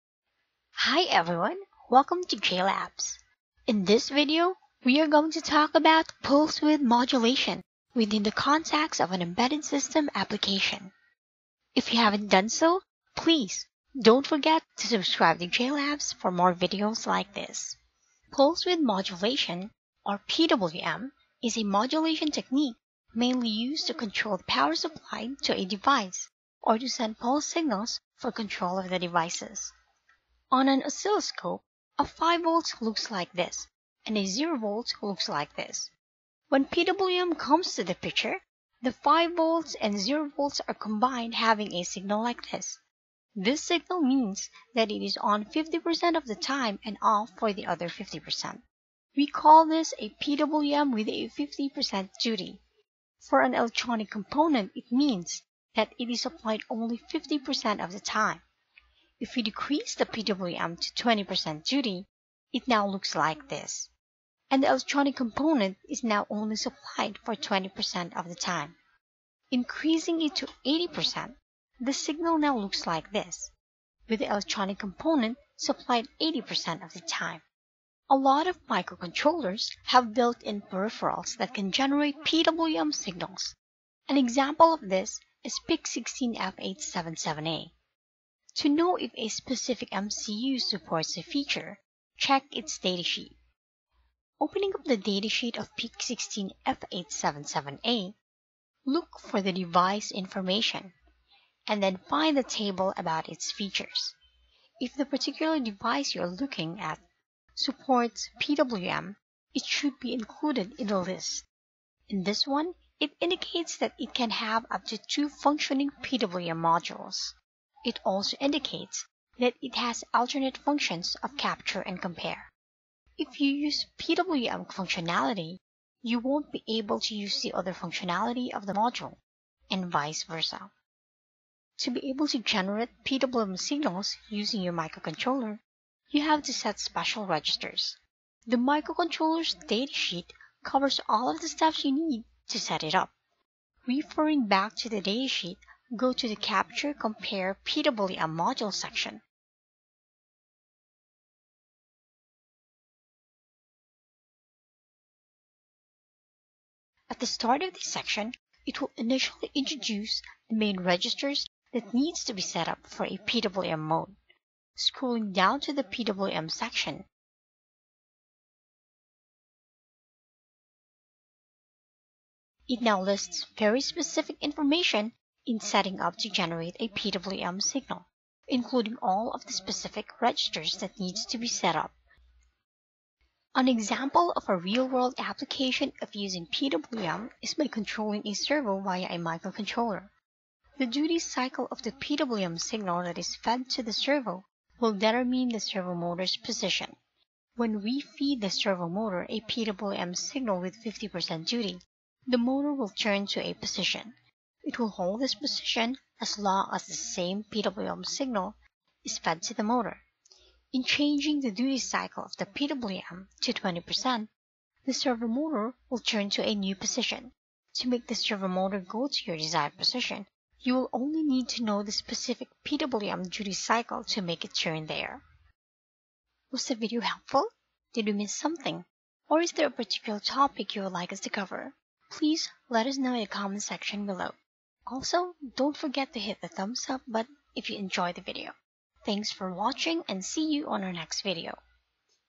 What is Pulse Width Modulation (PWM)?
Pulse Width Modulation is a modulation technique in supplying power to a device.